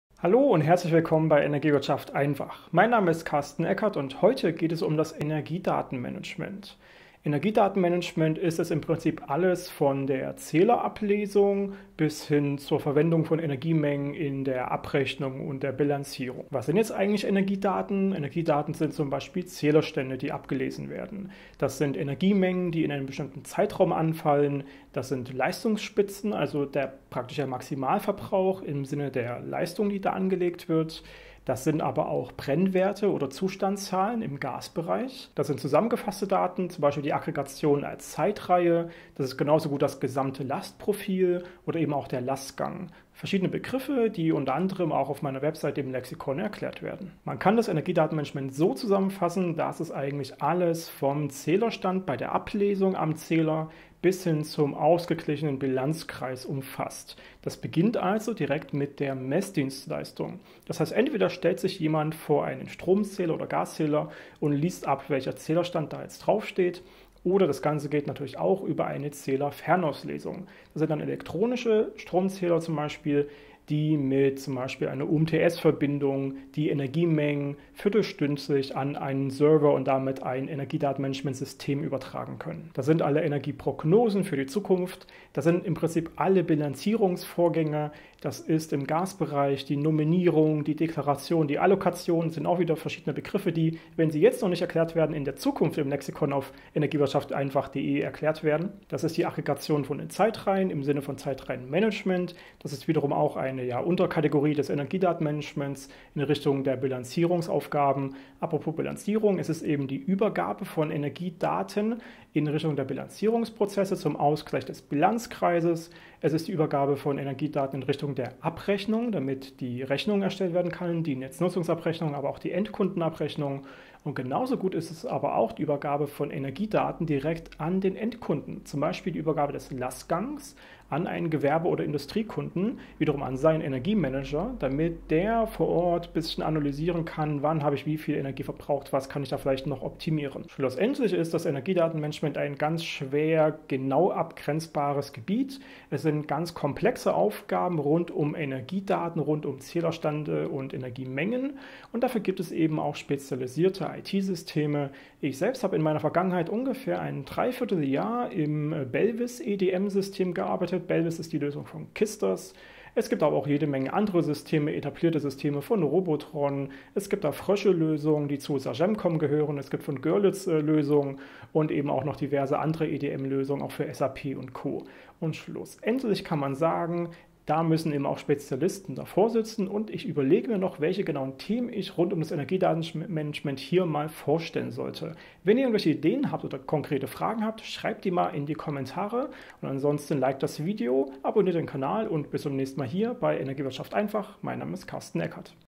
Energiedatenmanagement - Überblick
Was ist Energiedatenmanagement? In diesem Video geht es darum, was Energiedaten sind und was das EDM genau umfasst?

Welche Fragen habt ihr zum Energiedatenmanagement?